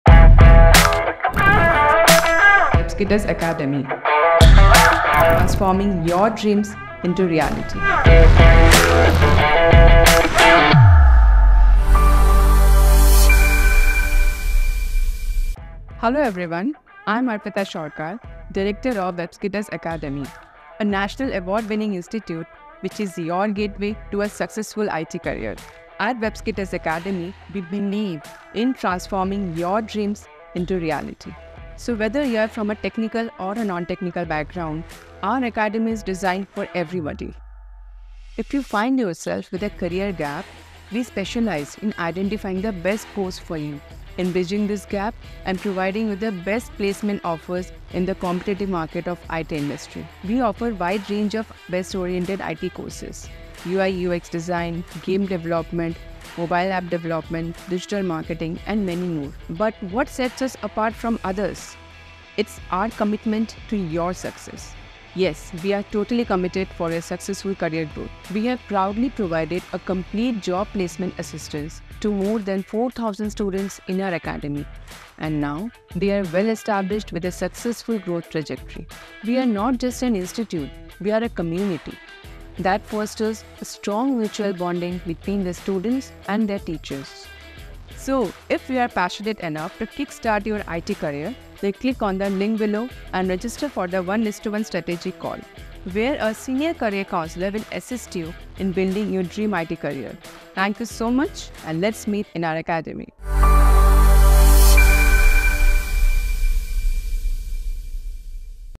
Transform Your Dreams Into Reality At Webskitters Academy | Best IT Institute In Kolkata
Step into Webskitters Academy, where we're all about making dreams come true! Our nationally recognized and award-winning IT institute, located in Kolkata, is known nationwide for its top-notch IT training. No matter where you're from or what you know, we've got courses that fit you just right.

Got a gap in your resume? No problem! Our job-focused placement help is here to connect you with great jobs.

From cool UI/UX Design to fun Game Development, from making Mobile Apps to rocking Digital Marketing and Web Design, we've got something for everyone. We've helped over 4000+ students reach their goals, and we'd love to help you too!

Let's team up and turn your dreams into reality at Webskitters Academy!